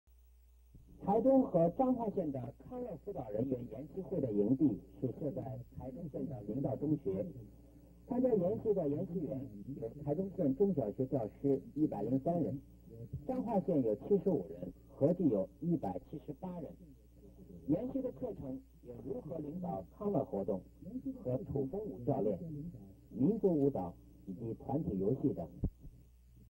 台中彰化康樂輔導人員研習會
(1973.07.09)台中和彰化縣康樂輔導人員研習會，在台中縣的明道中學舉行